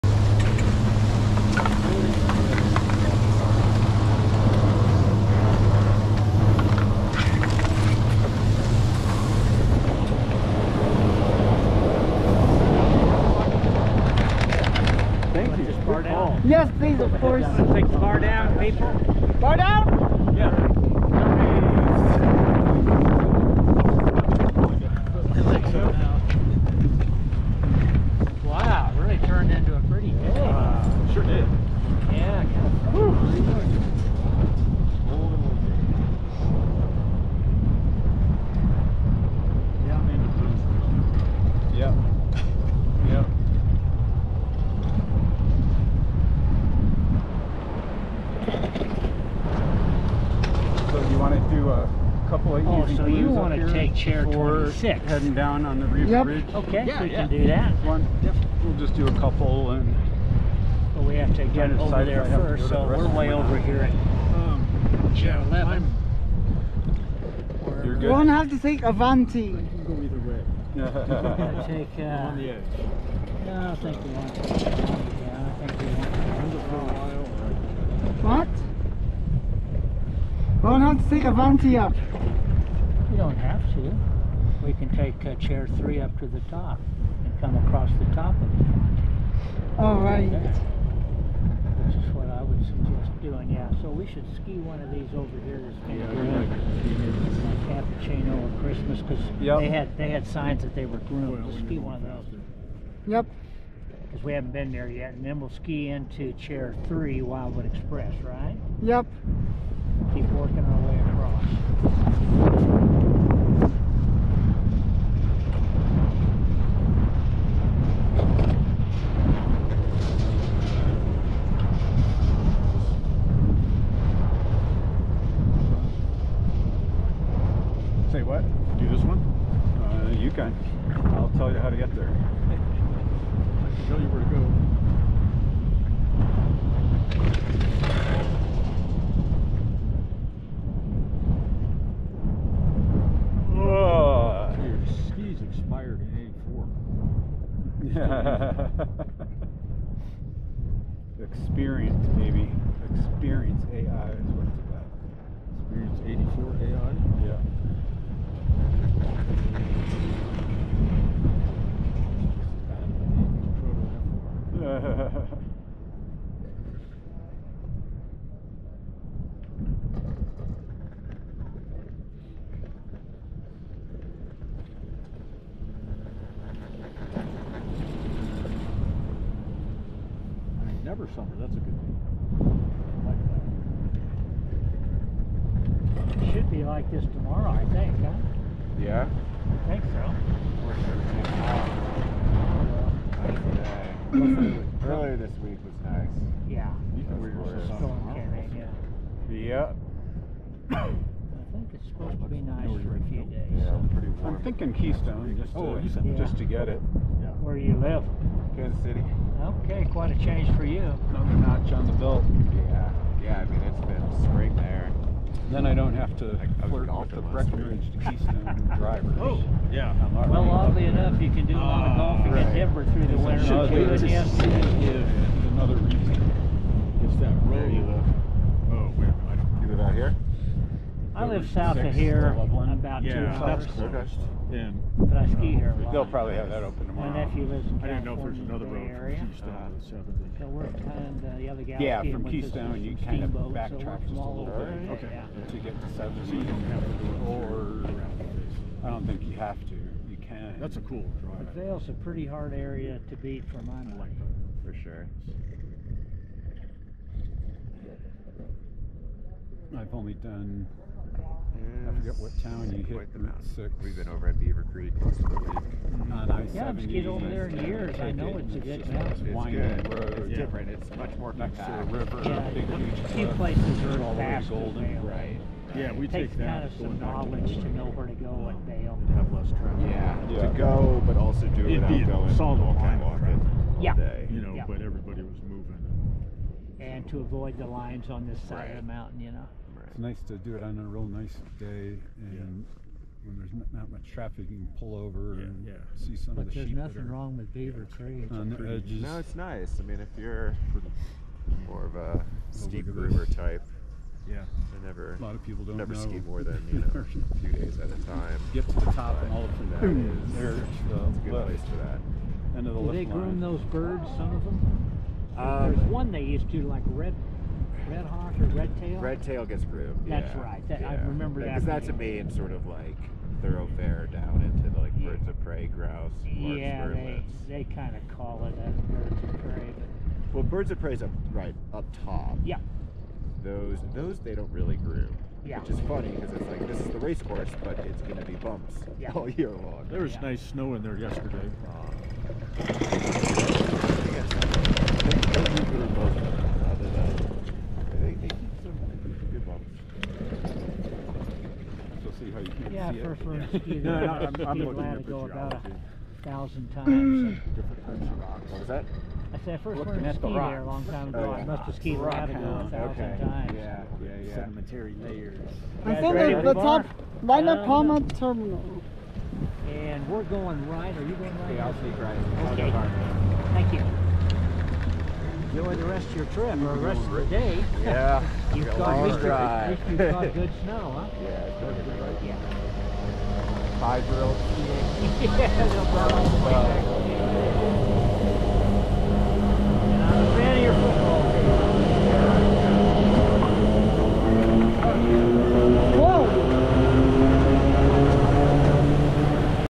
(3/15/24) Vail Mountain - Northwoods Express Lift 11 (4K60fps)
Northwoods Express was built by Leitner-Poma in 2017 to replace an old-school 1985 Doppelmayr high speed quad. Lift 11 has LPA terminals and grips with Leitner chairs imported from Europe. This lift has a capacity of 3,000 people per hour and was the first Leitner-Poma high speed six at Vail because Doppelmayr completed Lifts 2 and 4. Northwoods Express is 5,905 feet long and 1,545 feet tall.